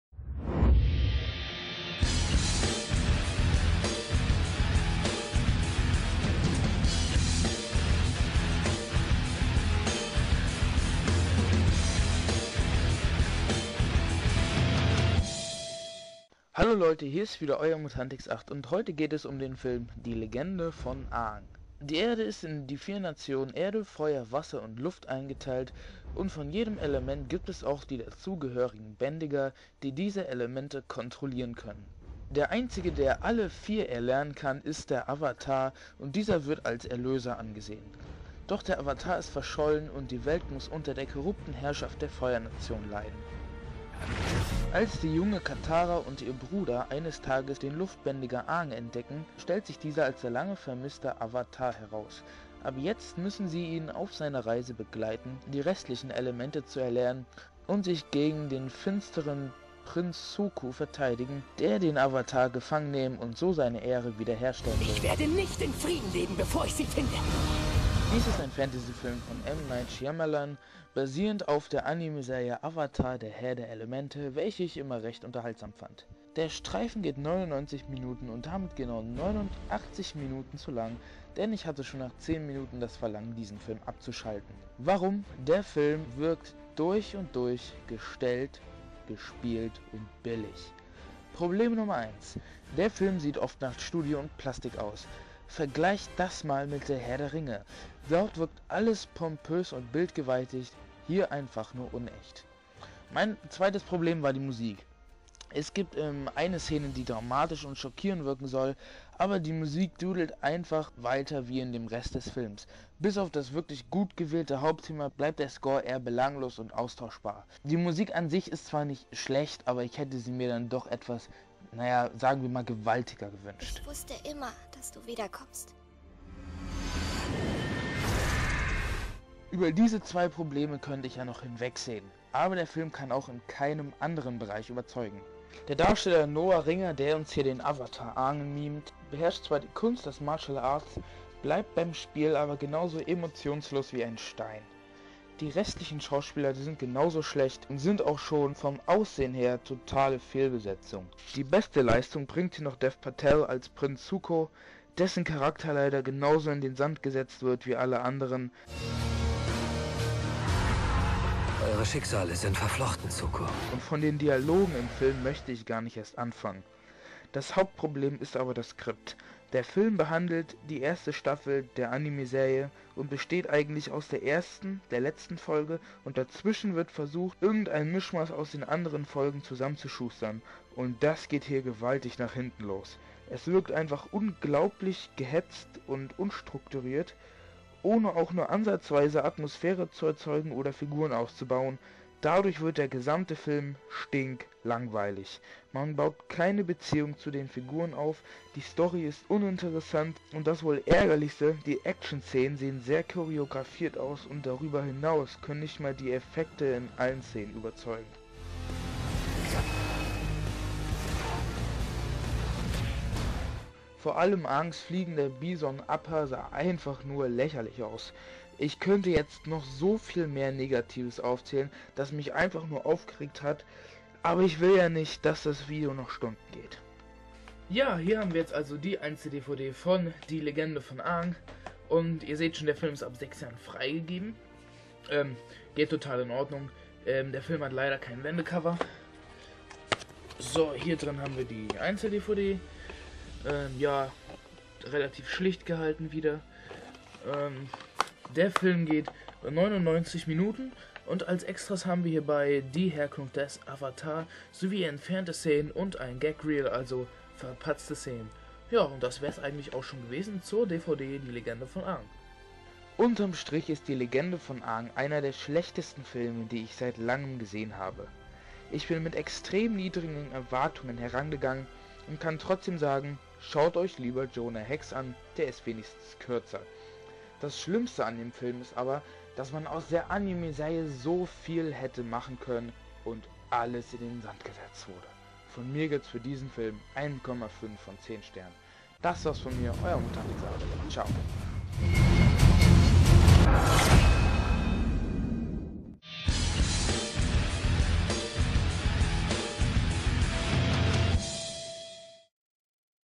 DIE LEGENDE VON AANG (Einzel DVD) Kritik Review
M. Night Shyamalan, bekannt durch werke wie "The Sixth Sense", "Unbrakeable" und "The Happening" versucht sich an der Realverfilmung von "Avatar - Der Herr der Elemente". Geht das gut? Nächste Woche zeigt uns Hayden Christensen wie gut er im Weitsprung ist.

Rechte bei:
Paramount